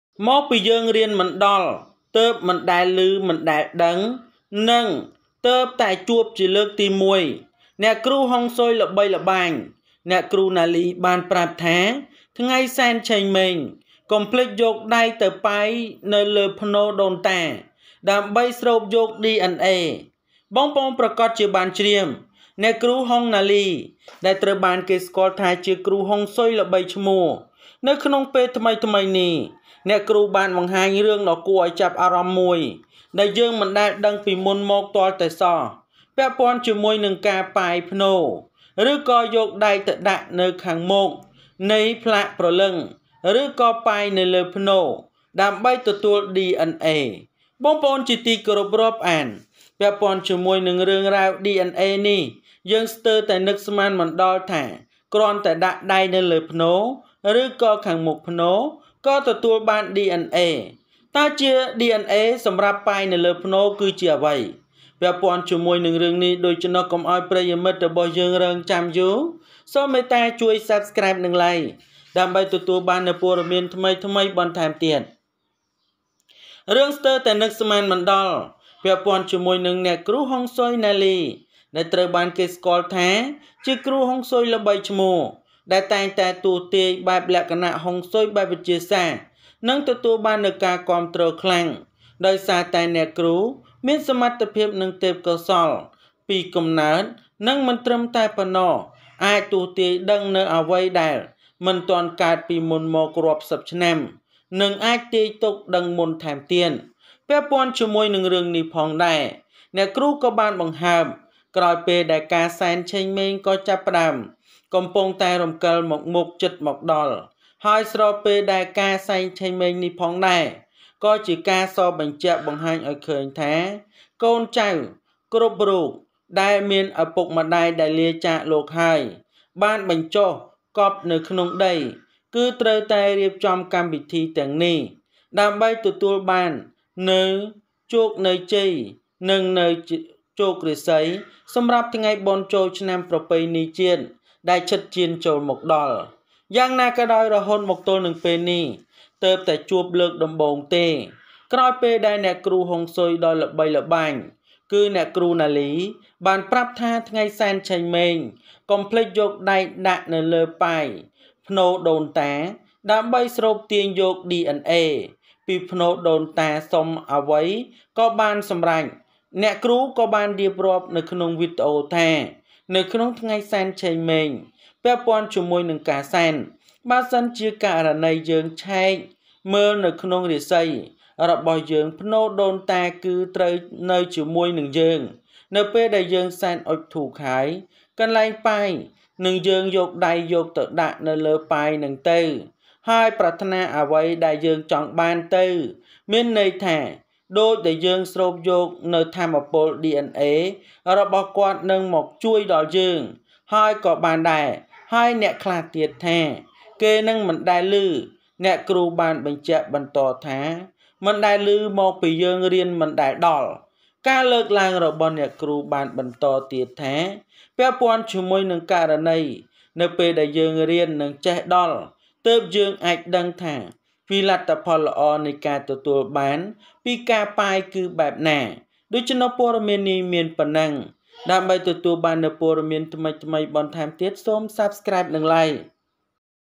គ្រូហុងស៊ុយដ៏ល្បាញល្បី ណាលី ប្រាប់ថា ថ្ងៃសែនឆេងម៉េងកុំភ្លេចយកដៃដាក់លើប៉ាយផ្នូដូនតា ដើម្បីស្រូបយកDNA
គ្រូហុងស៊ុយដ៏ល្បាញល្បី ណាលី ប្រាប់ថា ថ្ងៃសែនឆេងម៉េងកុំភ្លេចយកដៃដាក់លើប៉ាយផ្នូដូនតា ដើម្បីស្រូបយកDNA ពីផ្នូរដូនតាសុំ Famous feng shui teacher Naly says that on Sen Cheng Meng day, do not forget to put your hand on the ancestral tomb to absorb DNA from the ancestral tomb.